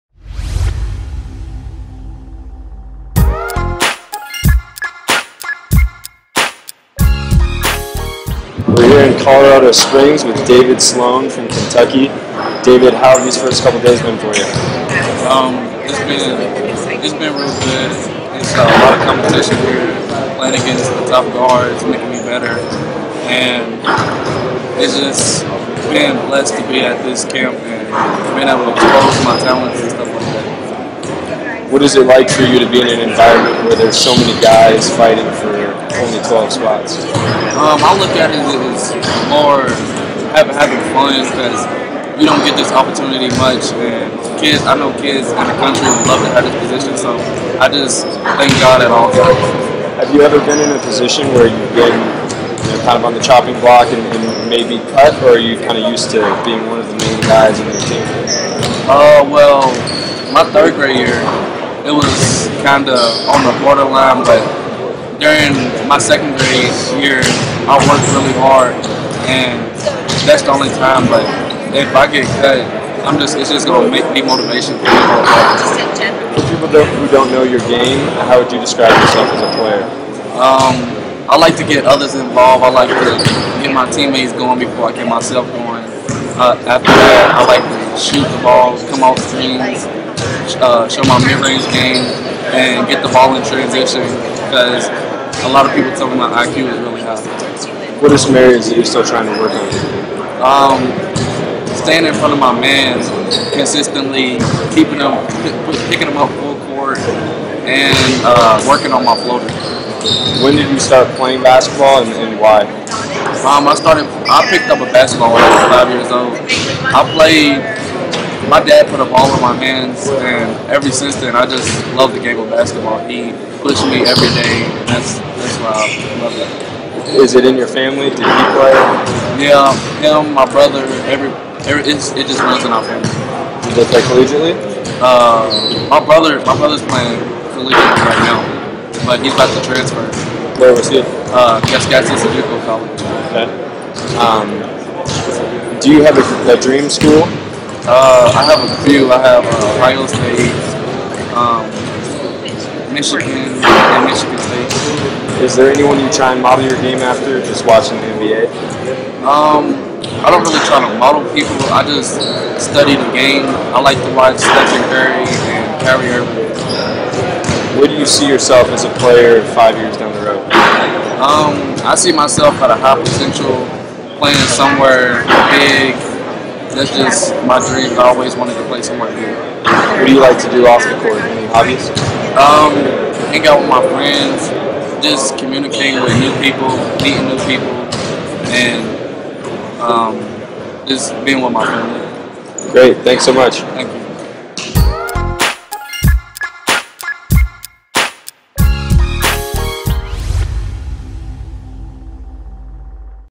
David Sloan 2015 Team USA U16 Interview - DraftExpress
Point guard David Sloan sits down with DraftExpress at the 2015 Team USA U16 Training Camp in Colorado Springs.